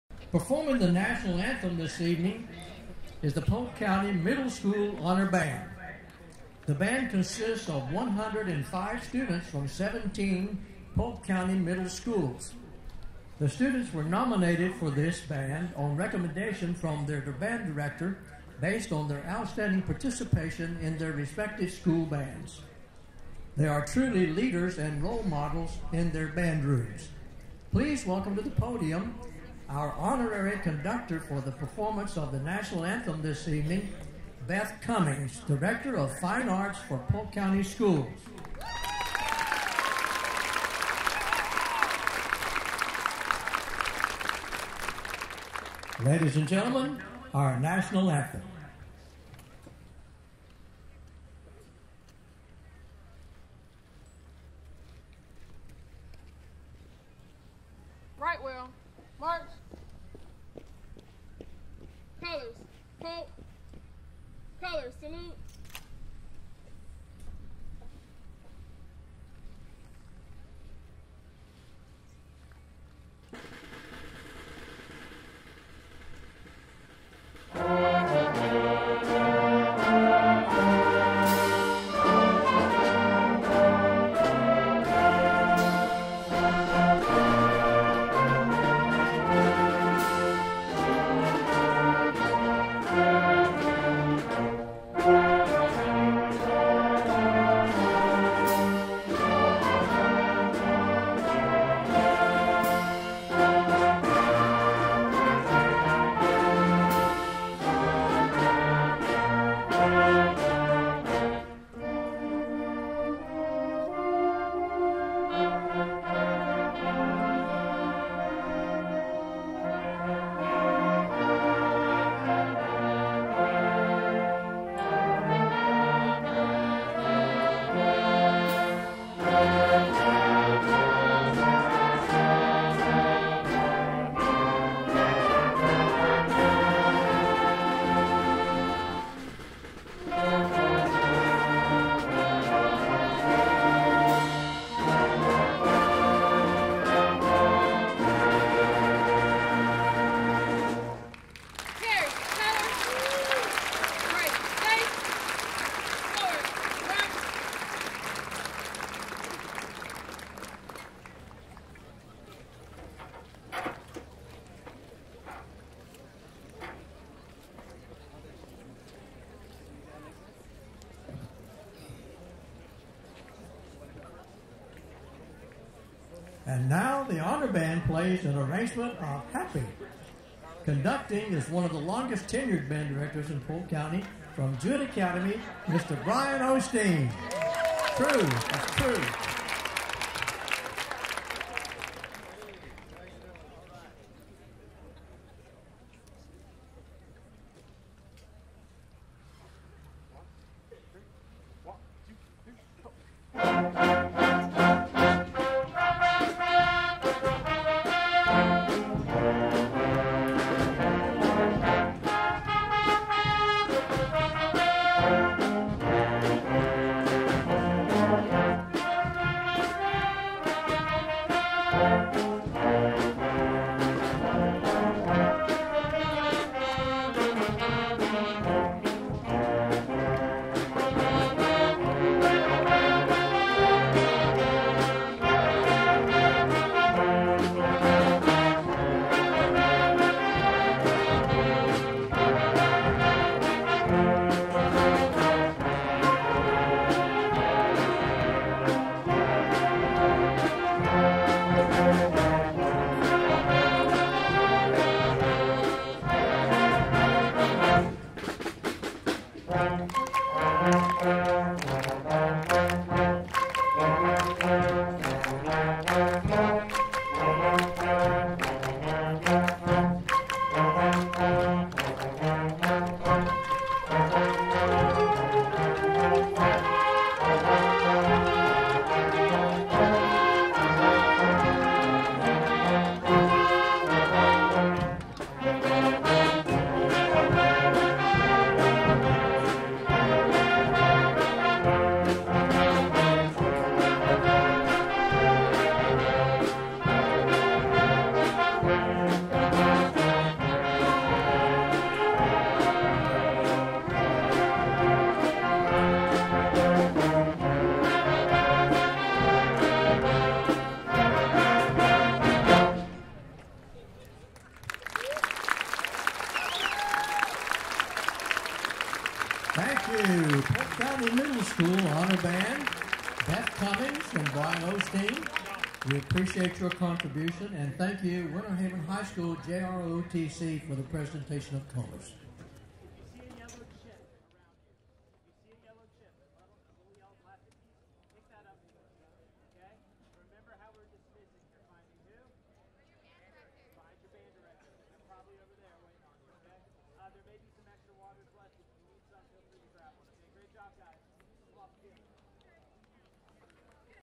Polk County Middle School Honor Band - Opening Performance @ MPA 2017 [HD]
We are very proud of our Polk County Middle School Students performing in the 2017 Honor Band! These up-and-coming musicians are the future of fine arts in our great district. Many thanks goes out to all Polk County Middle School Band Directors for allowing their students this opportunity!

►HONORARY CONDUCTOR: Beth Cummings
►COORDINATOR: Bryan O'Steen

FLORIDA BANDMASTER'S ASSOCIATION DISTRICT 12 MPA:

►HOSTS: Daniel Farr & Kasey Rakes
►ANNOUNCER: Jim Parker

►JUDGES:
      • Visual Performance - Steve DeLadurantey
      • General Effect - Jody Dunn
      • Auxiliary - Mary White

►ACKNOWLEDGEMENTS:
      • FBA President - Jason Duckett
      • FBA Executive Director - Neil Jenkins
      • FBA District 12 Chairman - Zachary Murdock
      • FBA District 12 Secretary - Daniel Farr

      • Polk County Schools Superintendent - Jacqueline Byrd
      • Polk County Schools Associate Superintendent/Chief Academic Officer - Dr. Michael Akes
      • Polk County Schools Director of Fine Arts - Beth Cummings
      • Polk County Schools Instrumental Teacher Resource Specialist - Chuck Fulton

      • Billy Townsend, Polk County School Board District 1
      • Lori Cunningham, Polk County School Board District 2
      • Hazel Sellers, Polk County School Board District 3
      • Sara Reynolds, Polk County School Board District 4
      • Kay Fields, Polk County School Board District 5
      • Lynn Wilson, Polk County School Board District 6
      • Tim Harris, Polk County School Board District 7